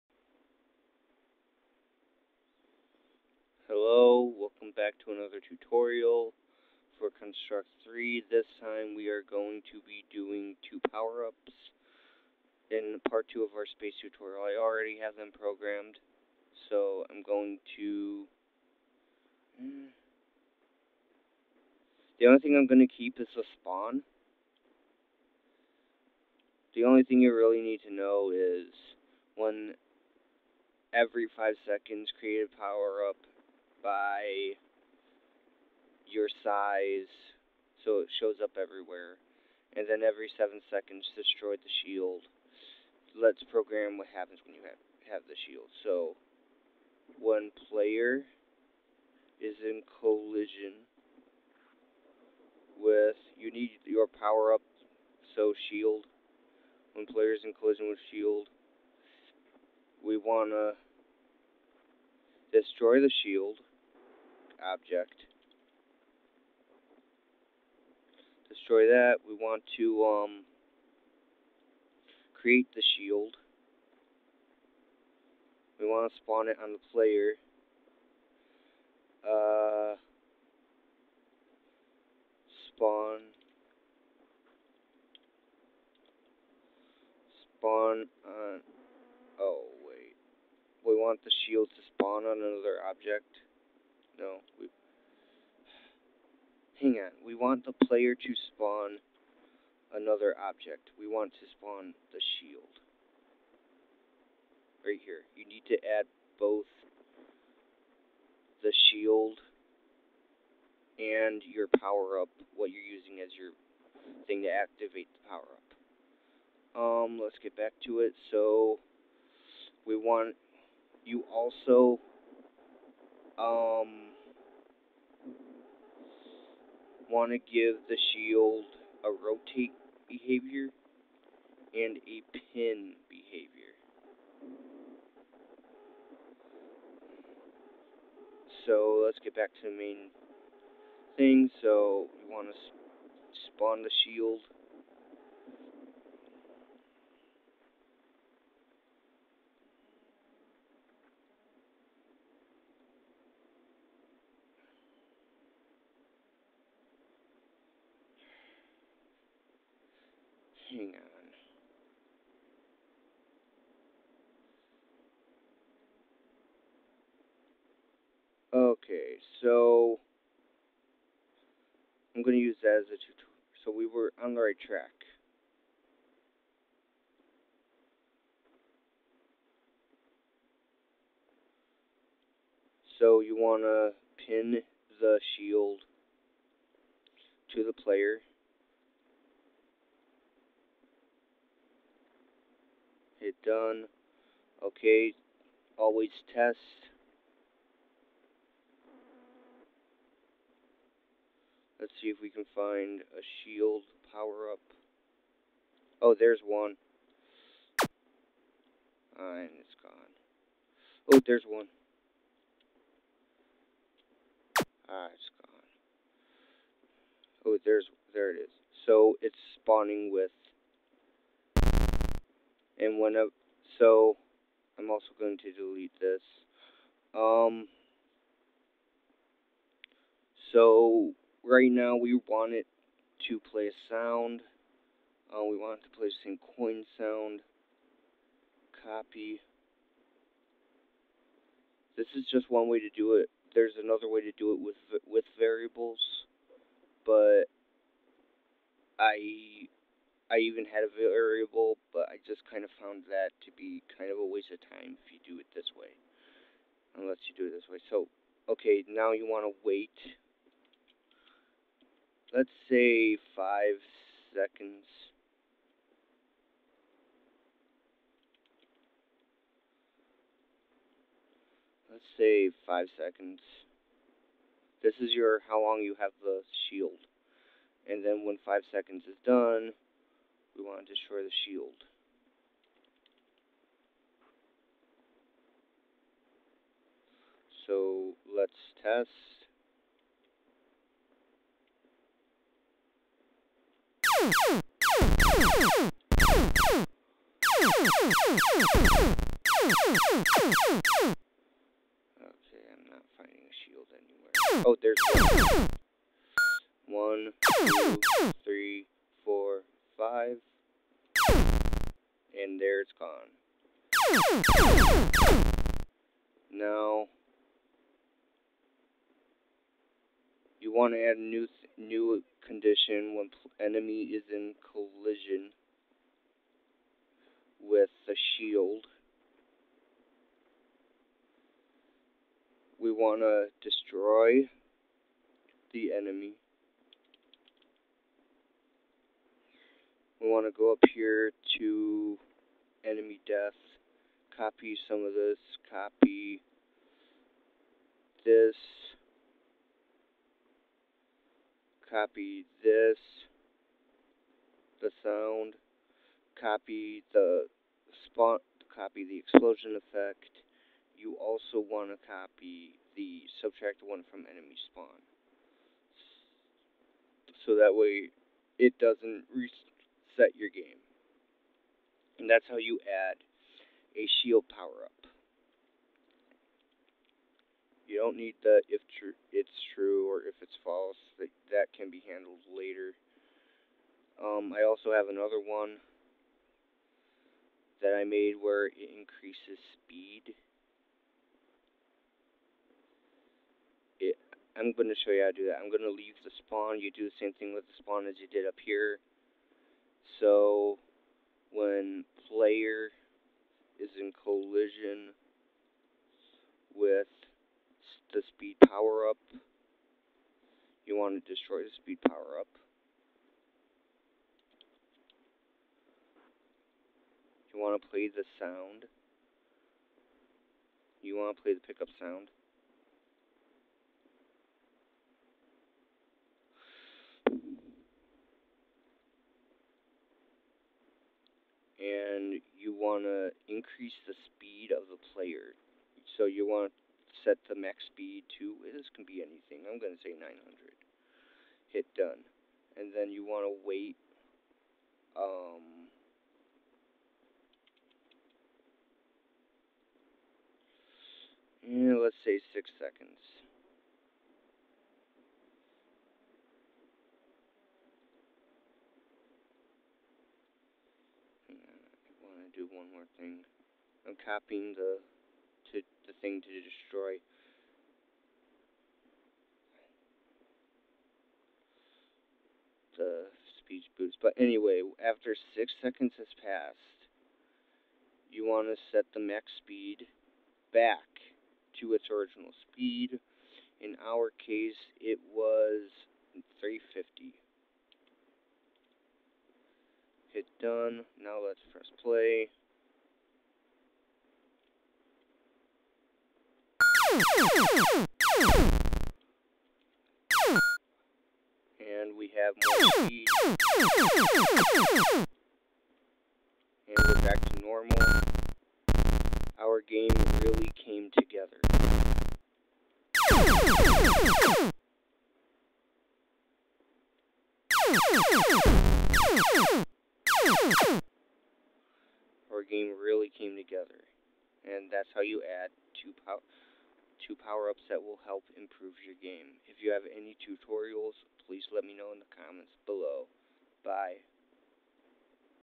Construct 3 tutorial: Powerups
In this tutorial, I added two powerups.